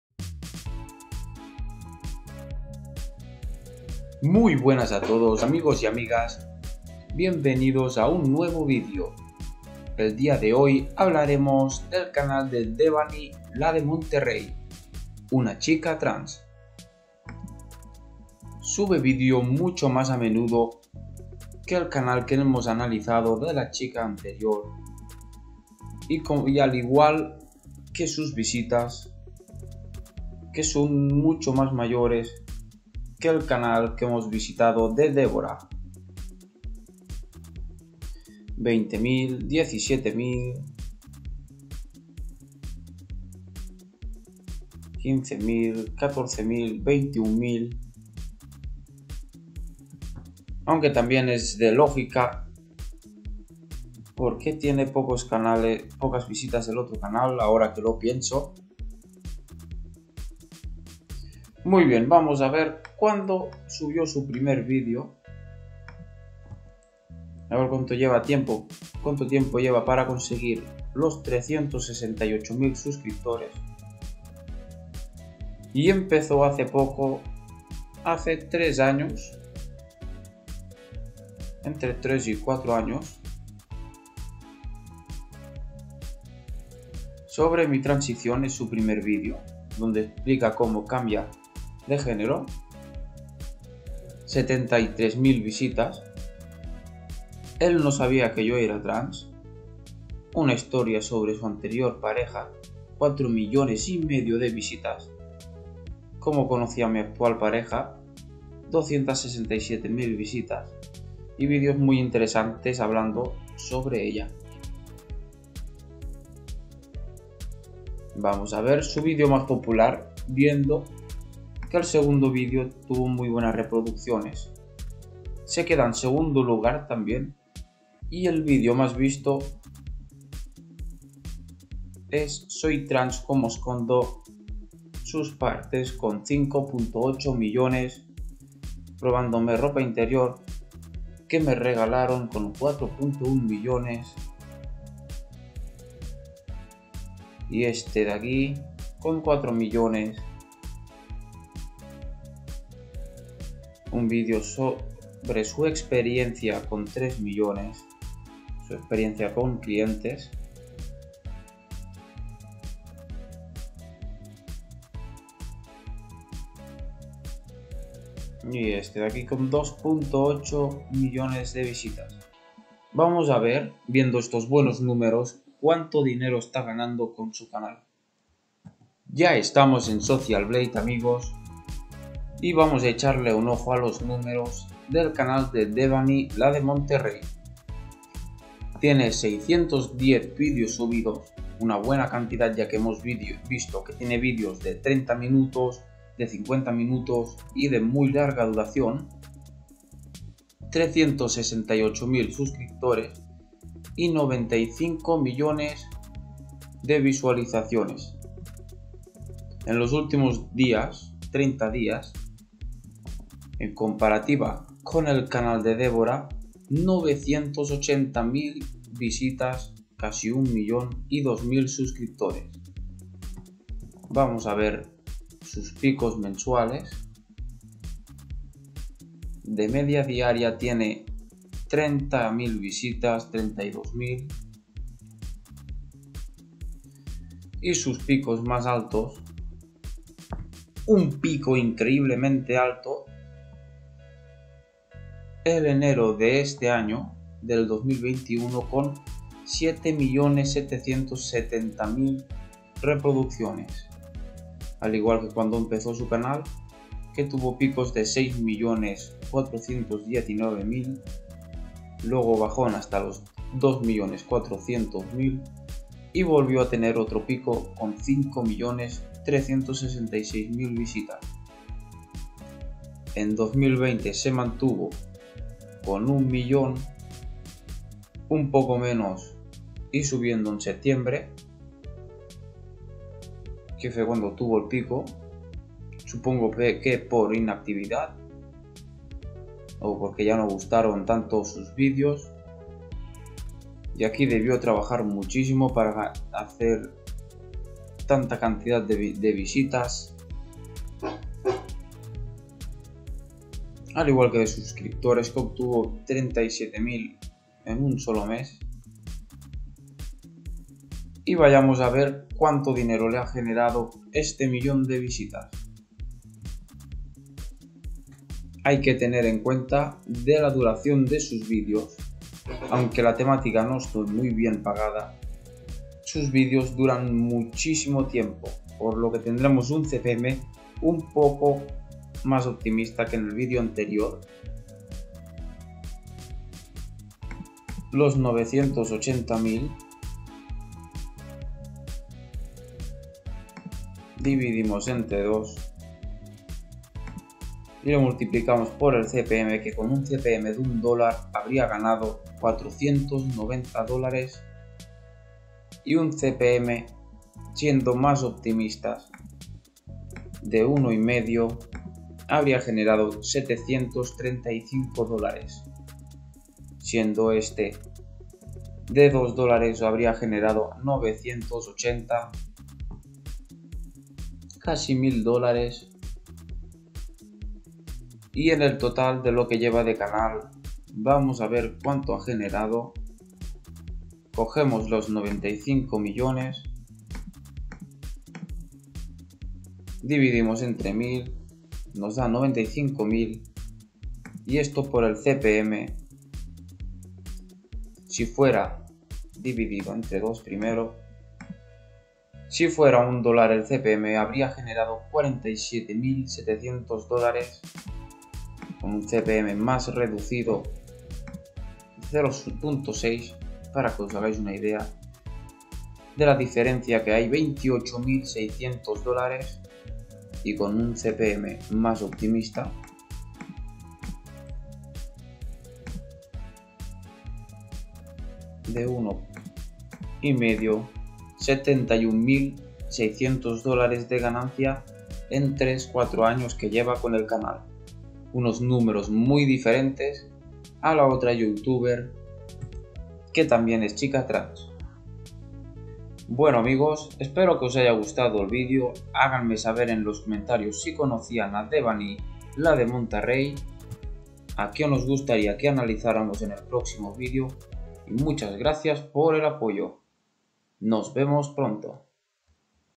🟩🤑 CUANTO GANA DEVANY LA DE MONTERREY EN YOUTUBE
Cuanto dinero gana Devany La De Monterrey al mes en YouTube. Descubre Como gana dinero devany y sus ingresos al año.

Indice
00:00 - Introduccion y quien es
02:58 - Estadísticas y visitas
05:11 - Ingresos